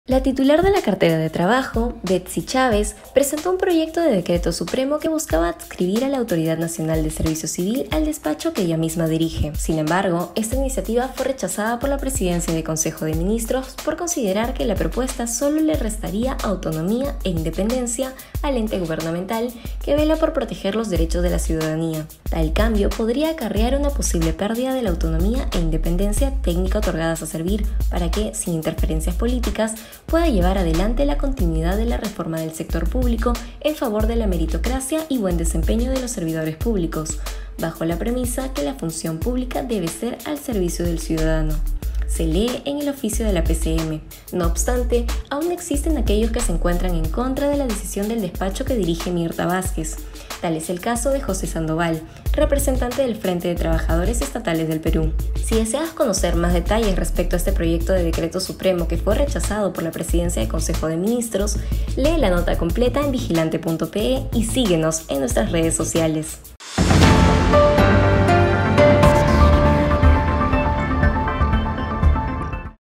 Servir: PCM rechaza iniciativa de la ministra Betssy Chávez | Vigilante
El oficio emitido por la Presidencia del Consejos de Ministros afirma que el cambio propuesto por Chávez le restaría autonomía e independencia al ente gubernamental